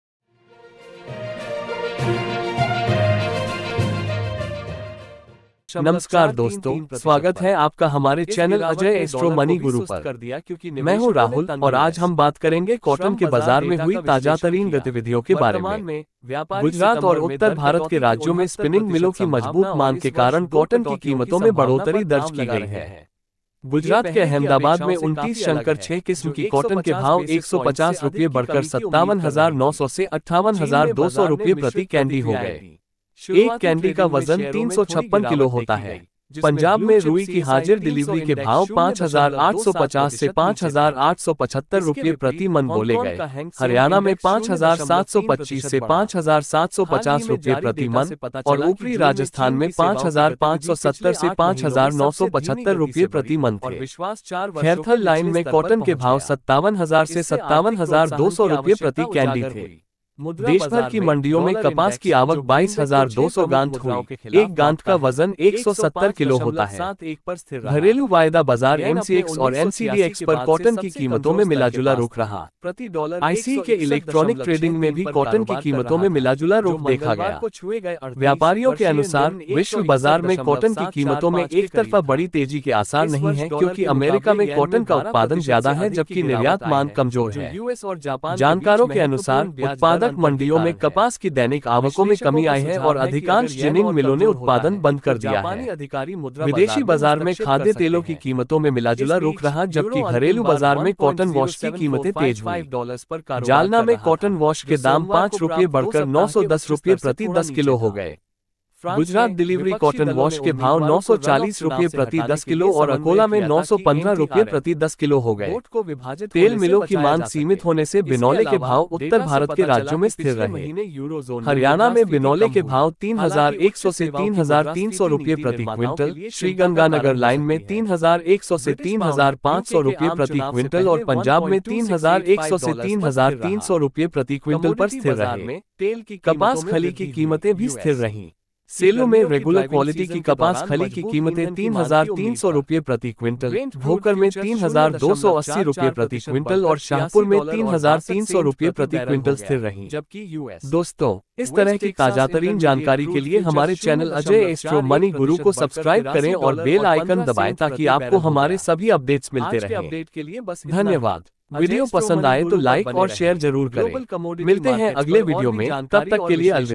कपास बाजार में तेजी | जानें क्या है कारण?
सरसो तेल व राइस ब्रान तेल में बनी तेजी, अग्रिम अनुमान सही साबित। कर्नल अजय की रिपोर्ट

हल्दी के आयात में अप्रैल महीने में 6 गुना की वृद्धि, कर्नल अजय की विशेष रिपोर्ट Tuermeric Import